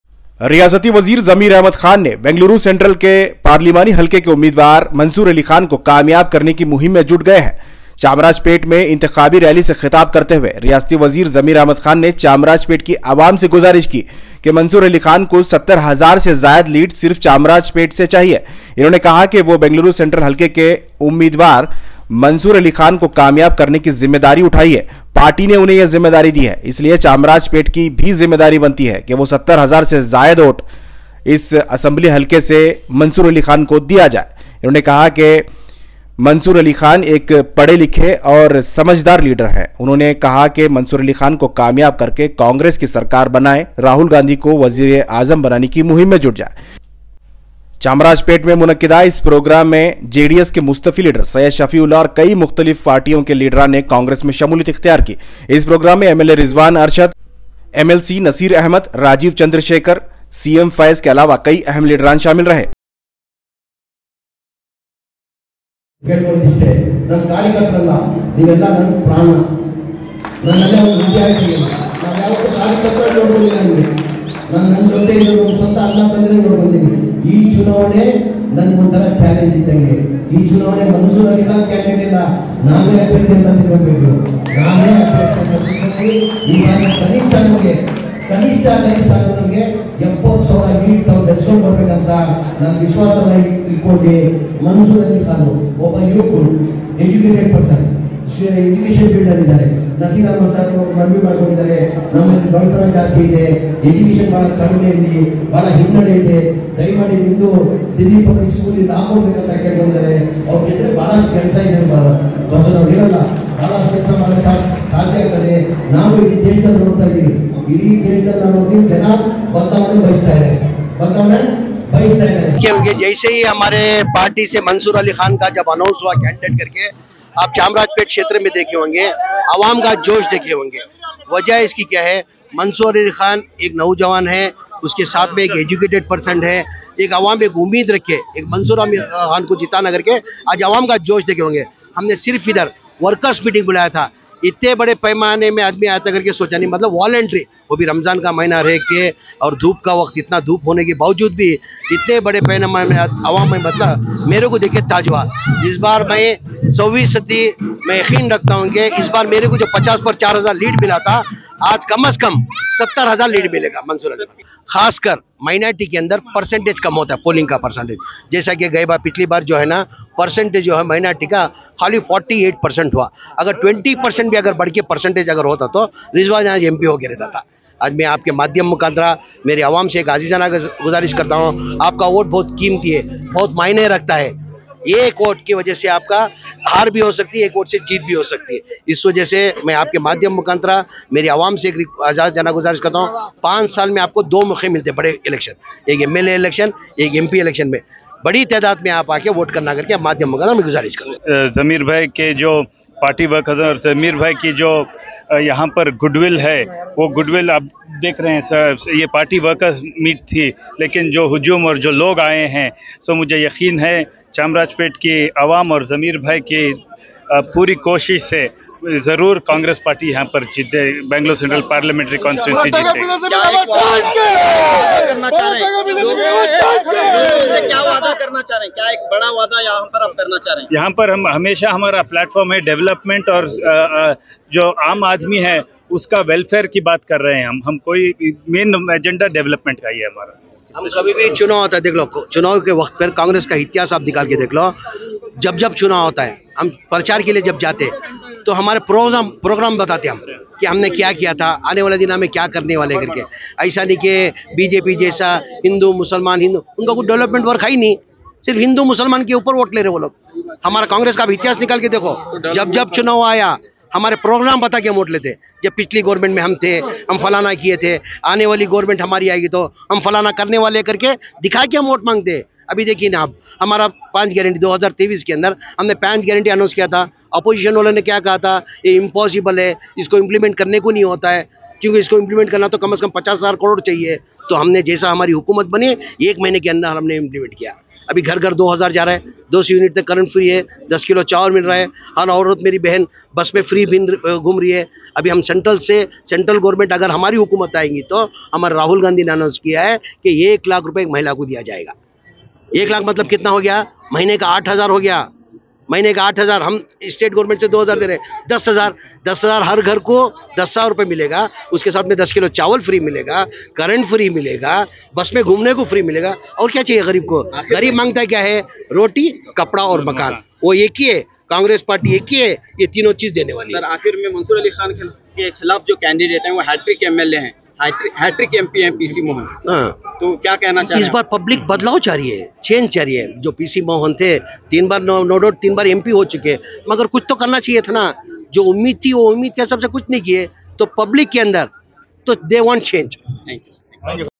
Minister Zameer Ahmed: All praises to the young and dynamic leader Mansoor Ali Khan.
Bangalore Central | Chamrajpet rally | BZ Zameer Ahmed khan | Mansoor ali khan